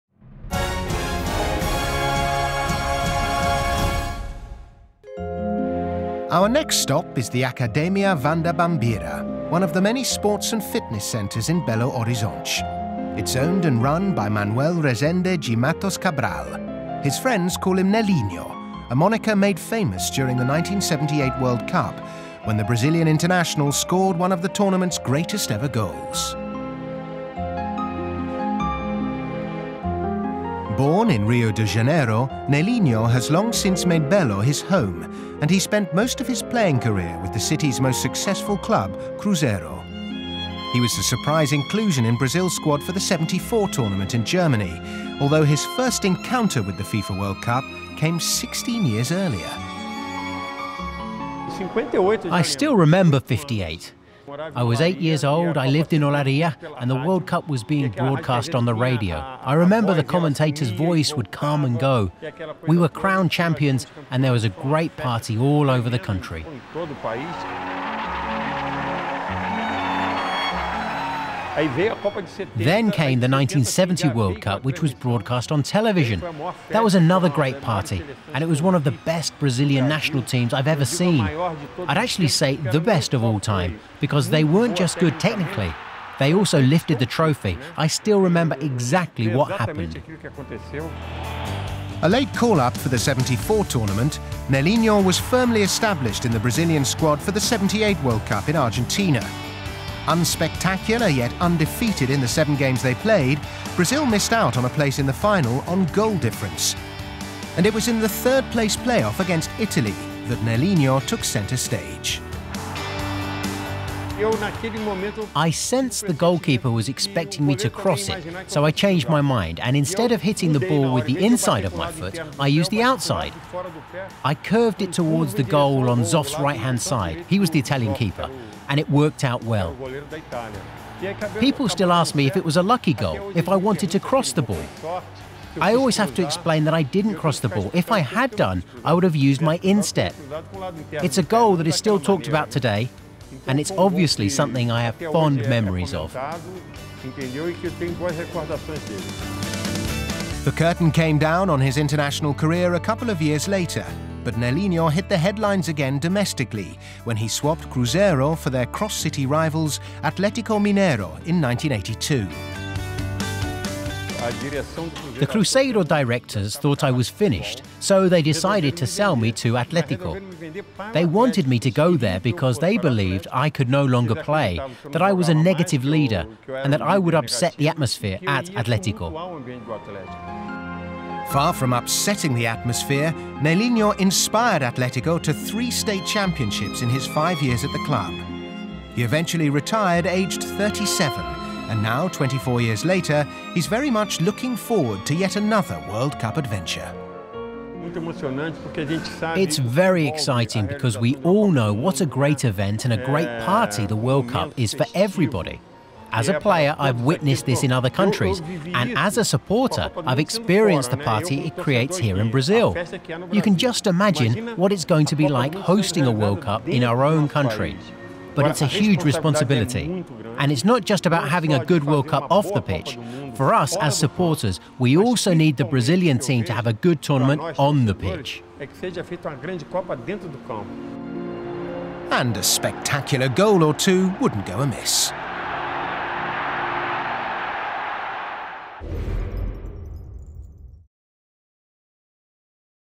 Nelinho: Famous goal was a shot, not a cross
The Brazil 2014 Magazine talks with Nelinho, a Brazilian great, who scored one of the most impressive goals in FIFA World Cup history in the 1978 Match for third place against Dino Zoff and Italy.